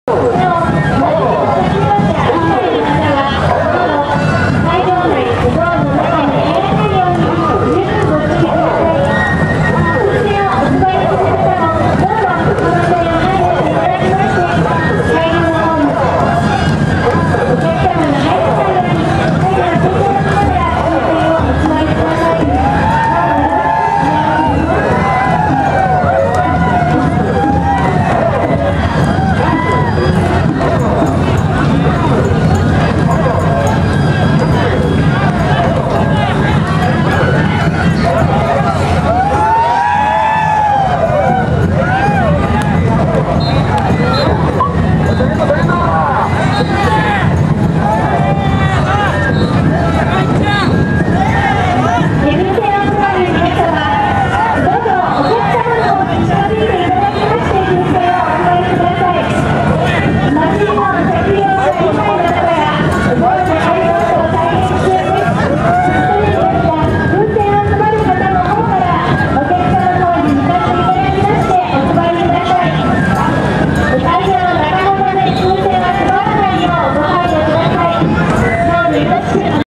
まつりつくば2011.8.27 獅子舞
2010年8月27・28日茨城県つくば市で「まつりつくば2011」が開催されました。本動画は土浦学園線を封鎖して設けられたパレード会場での獅子舞の様子です。つくば西武と三井ビルを結ぶ歩道橋下から撮影しました。